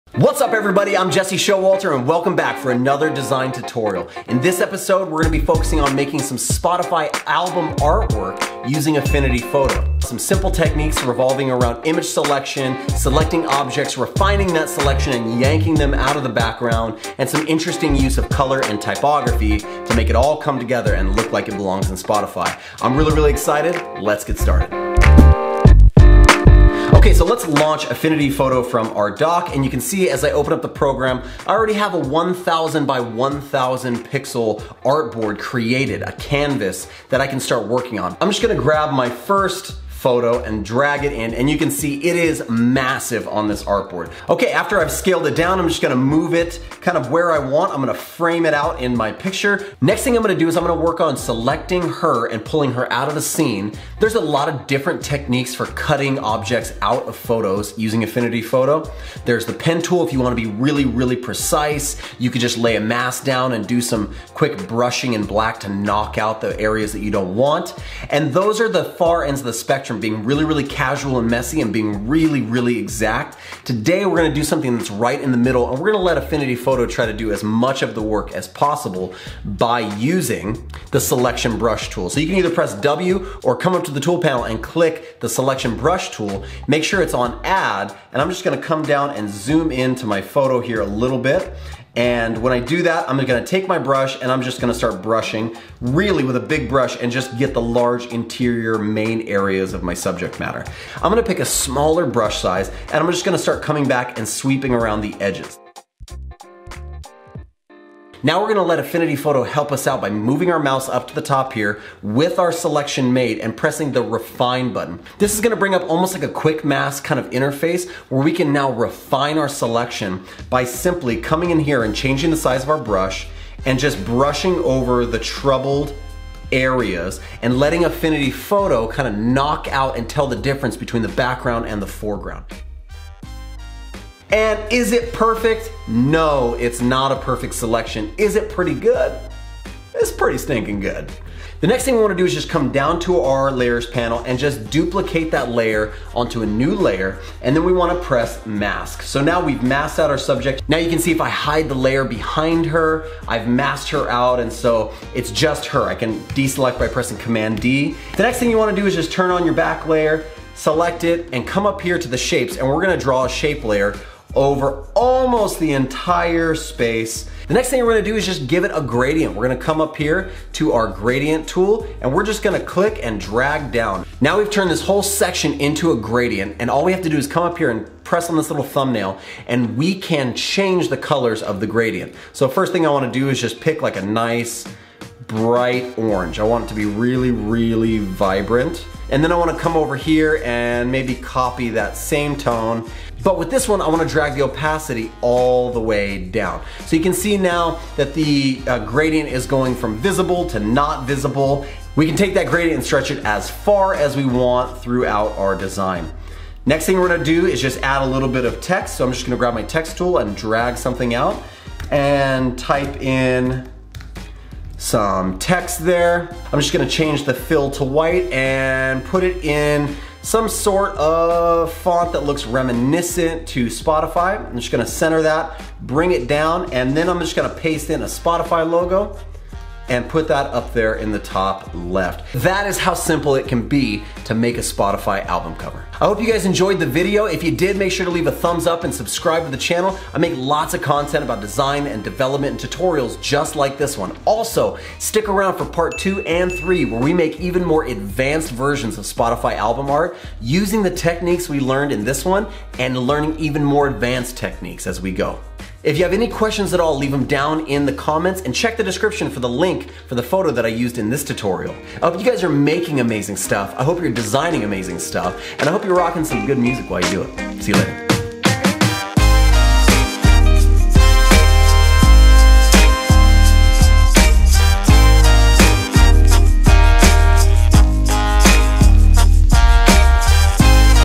Spotify Album Art Design Tutorial Part 1 - Affinity Photo Tutorial
In today's design tutorial, we recreate some Spotify Album Art in Affinity Photo.

If you want to download the photo I am using for this project by Alexandra Kirr and check out here work

Spotify has created not only a fantastic music streaming service but they have also carved out for themselves a pretty distinct style of design in their album and playlist covers.

Popularized by their "Duotone Effect"(which we will be doing in a later video) Spotify has done a great job at standing out and creating a consistent style across the board.

Whether you are using a different program than Affinity Photo (perhaps something like photoshop) it doesn't matter because the methods are the same to create Spotify album art.